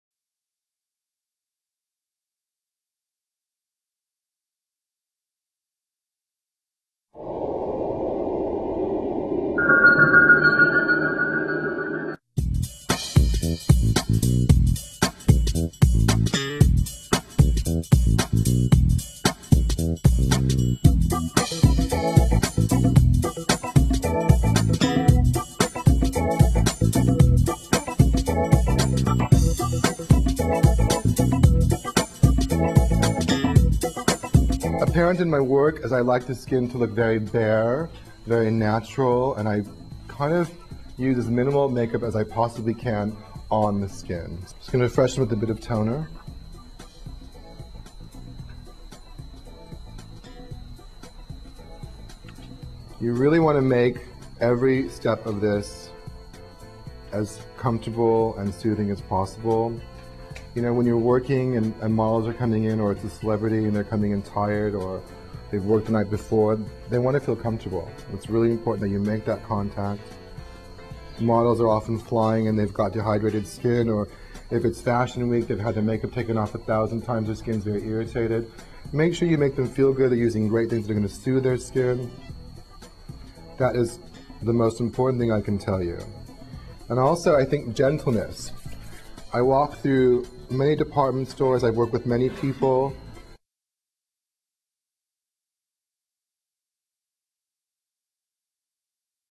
Bodyography | Ipod Series x 4 | Beauty & Cosmetic Tutorial | Goffrey Rodriguez
Educational Beauty  & Cosmetics Video for Bodyography | Makeup Artist: Geoffrey Rodriguez | Producer: Jane S. Linter for SZTV Media Group